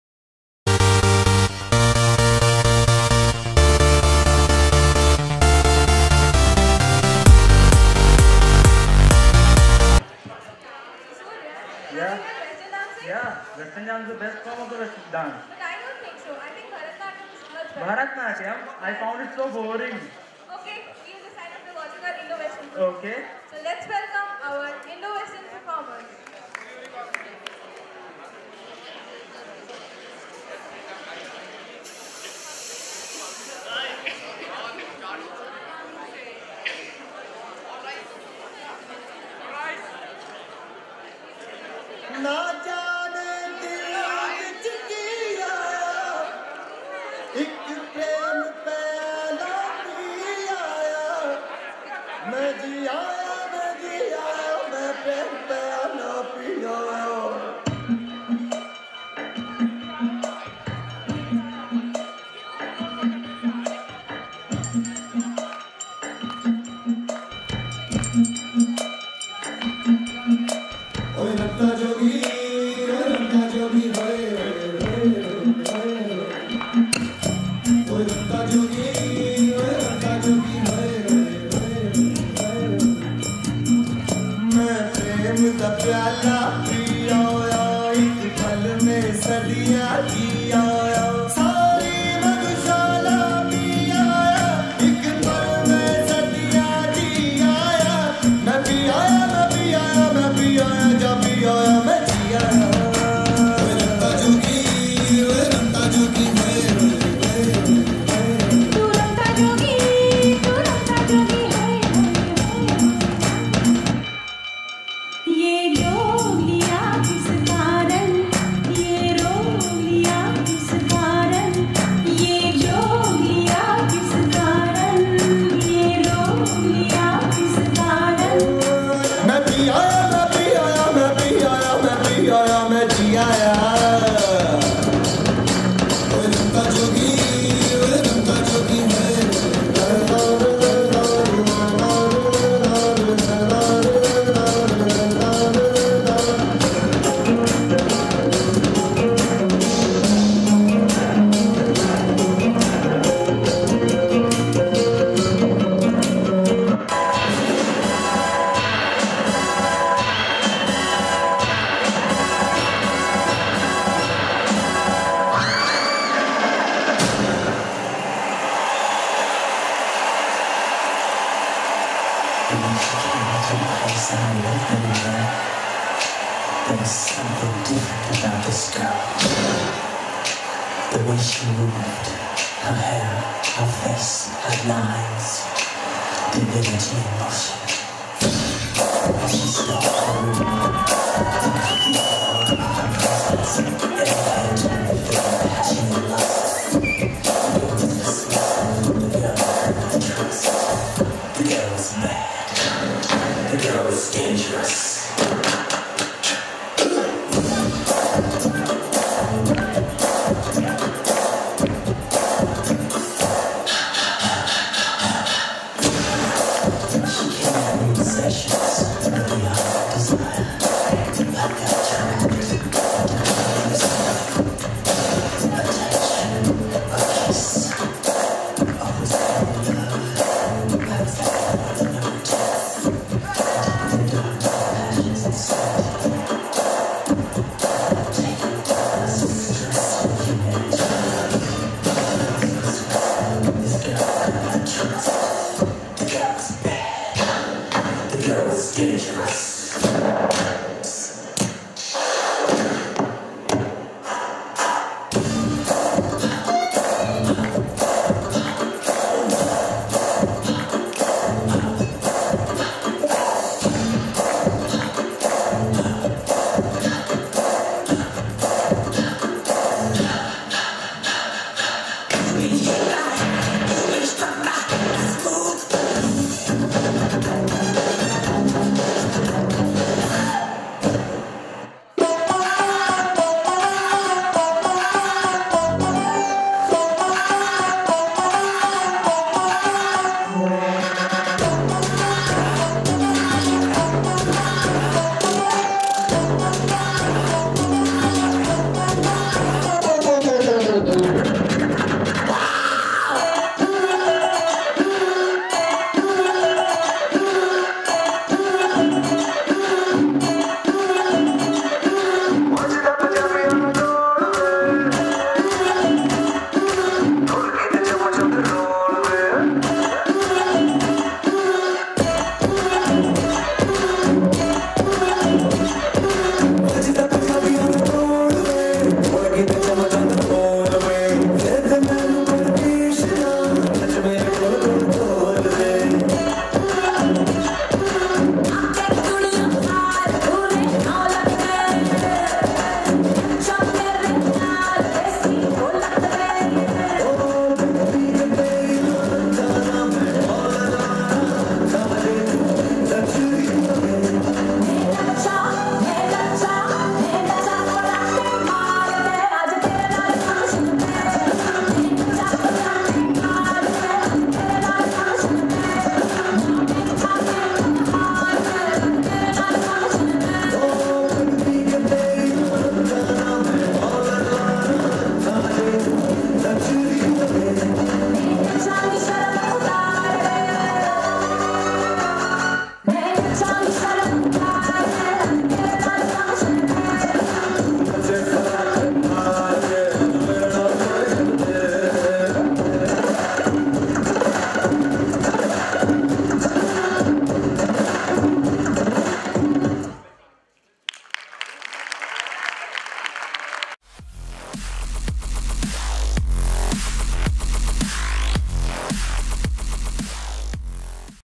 Fresher's Party 2k15: BITM (Telecom & Marketing) PURAB-PASCHIM
Student Managers of BITM (Telecom & Marketing) performing PURAB PASCHIM (INDO WESTERN) in BITM fresher'S party.